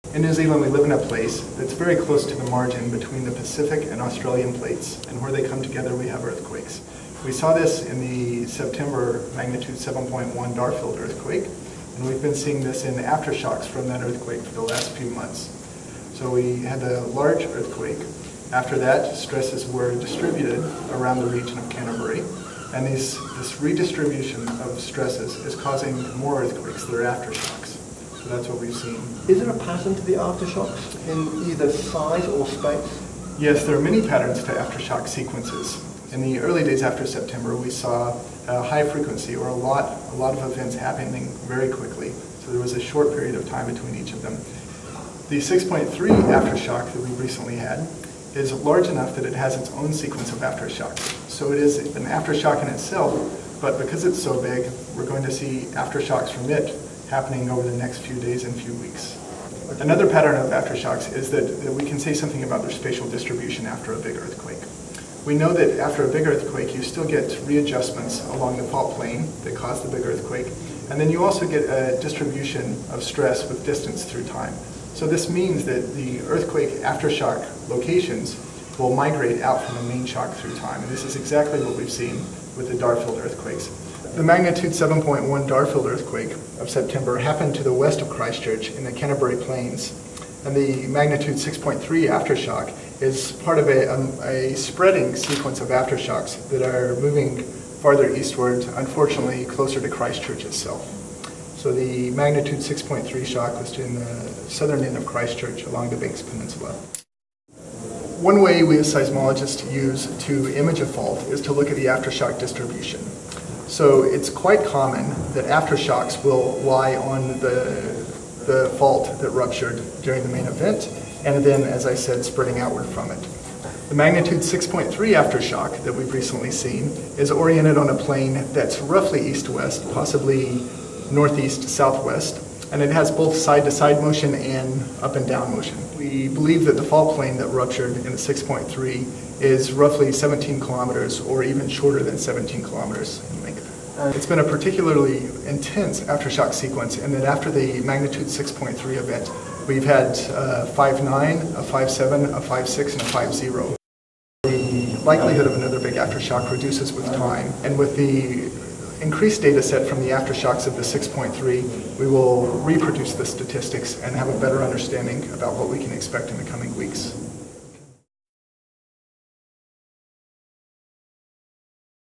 Christchurch February Aftershocks
GNS seismologist Bill Fry explains the February 2011 aftershocks in Christchurch to Julian Thomson..